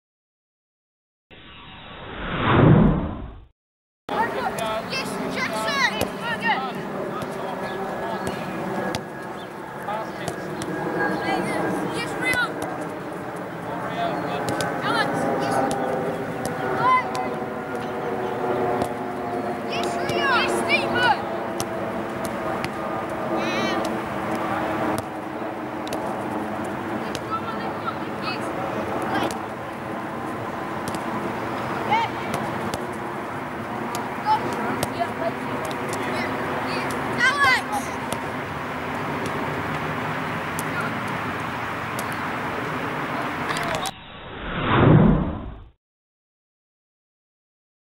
TOFFA Passing 01
TOFFA Kids hone their passing and movement with this morning passing exercise.